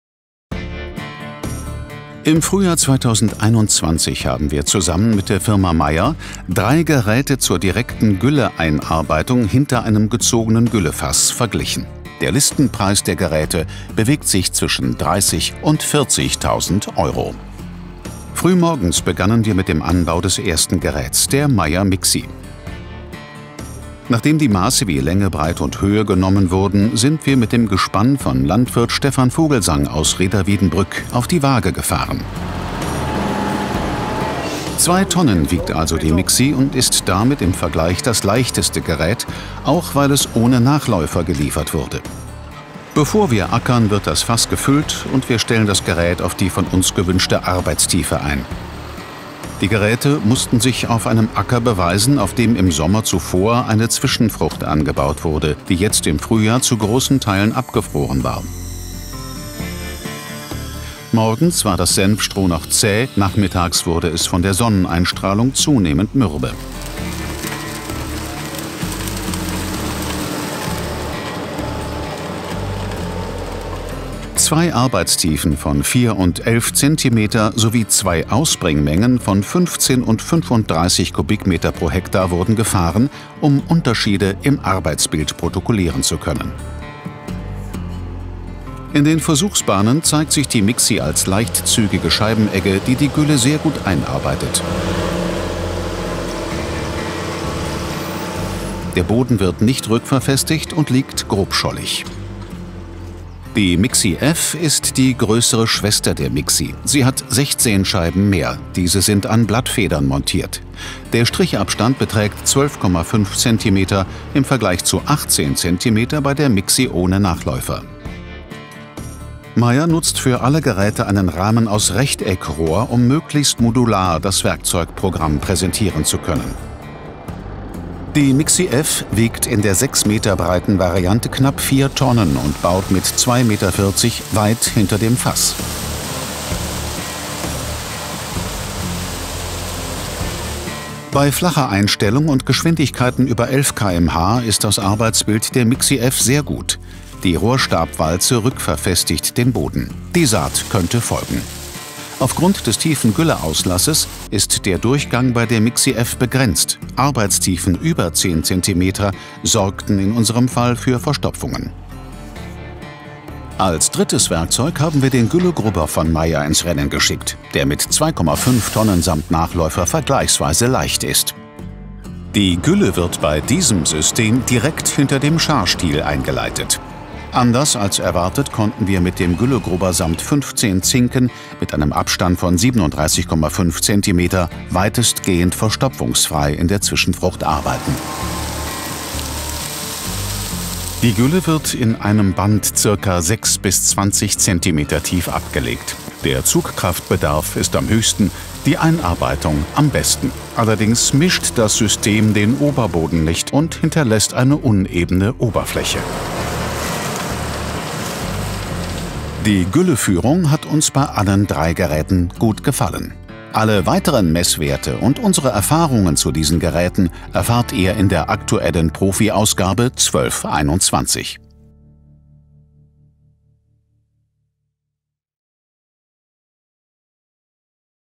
Gülleeinbringgeräte im Vergleich | profi
Zinken, Scheiben oder noch mehr Scheiben?
Mit welcher Technik lässt sich Gülle effektiv in den Boden bringen? Was sind Vor- und Nachteile verschiedener Scheibeneggen im Vergleich zum Grubber? Das haben wir mit den Geräten der Firma Meyer hinter einem gezogenen Güllewagen getestet.

Im Frühjahr 2021 haben wir zusammen mit der Firma Meyer drei Geräte zur direkten Gülleeinarbeitung hinter einem gezogenen Gülle-Fass verglichen.
Der Listenpreis der Geräte bewegt sich zwischen 30 und 40 Tsd. Euro.
Früh Morgens begannen wir mit dem Anbau des ersten Geräts: Der Meyer Mixi.
Nachdem die Maße wie Länge, Breite und Höhe genommen wurden, sind wir mit dem Gespann von Landwirt Stefan Vogelsang aus Rheda-Wiedenbrück auf die Waage gefahren.
2 t wiegt also die Mixi und ist damit im Vergleich das leichteste Gerät, auch weil es ohne Nachläufer geliefert wurde.
Bevor wir ackern, wird das Fass gefüllt und wir stellen das Gerät auf die von uns gewünschte Arbeitstiefe ein. Die Geräte mussten sich auf einem Acker beweisen, auf dem im Sommer zuvor eine Zwischenfrucht angebaut wurde, die jetzt im Frühjahr zu großen Teilen abgefroren war. Morgens war das Senfstroh noch zäh, nachmittags wurde es von der Sonneneinstrahlung zunehmend mürbe.

Zwei Arbeitstiefen von 4 und 11 cm, sowie zwei Ausbringmengen von 15 und 35 m³/ha wurden gefahren, um Unterschiede im Arbeitsbild protokollieren zu können.
In den Versuchsbahnen zeigt sich die Mixi als leichtzügige Scheibenegge, die die Gülle sehr gut einarbeitet. Der Boden wird nicht rückverfestigt und liegt grobschollig.
Die Mixi F ist die größere Schwester der Mixi. Sie hat 16 Scheiben mehr. Diese sind an Blattfedern montiert. Der Strichabstand beträgt 12,5 cm, im Vergleich zu 18 cm bei der Mixi ohne Nachläufer.
Meyer nutzt für alle Geräte einen Rahmen aus Rechteckrohr, um möglichst modular das Werkzeugprogramm präsentieren zu können.
Die Mixi F wiegt in der 6 m breiten Variante knapp 4 t und baut mit 2,40 m weit hinter das Fass.
Bei flacher Einstellung und Geschwindikeiten über 11 km/h ist das Arbeitsbild der Mixi F sehr gut. Die Rohrstabwalze rückverfestigt den Boden. Die Saat könnte folgen. Aufgrund des tiefen Gülleauslasses ist der Durchgang bei der MIXI F begrenzt. Arbeitstiefen über 10 cm sorgten in unserem Fall für Verstopfungen.
Als drittes Werkzeug haben wir den Güllegrubber von Meyer ins Rennen geschickt, der mit 2,5 t samt Nachläufer vergleichsweise leicht ist.
Die Gülle wird bei diesem System direkt hinter dem Scharstil eingeleitet.
Anders als erwartet konnten wir mit dem Güllegrubber samt 15 Zinken mit einem Abstand von 37,5 cm weitestgehend verstopfungsfrei in der Zwischenfrucht arbeiten. Die Gülle wird in einem Band ca. 6 bis 20 cm tief abgelegt. Der Zugkraftbedarf ist am höchsten, die Gülleeinarbeitung am besten. Allerdings mischt das System den Oberboden nicht und hinterlässt eine unebene Oberfläche.
Die Gülleführung hat uns bei allen drei Geräten gut gefallen.
Alle Weiteren Messwerte und unsere Erfahrungen zu diesen Geräten erfahrt ihr in der aktuellen profi Ausgabe 12/21.

Bericht: Tobias Bensing
Kamera: Michel Velderman
Schnitt: Roman Hünefeld
Sprecher: Christoph Wittelsbürger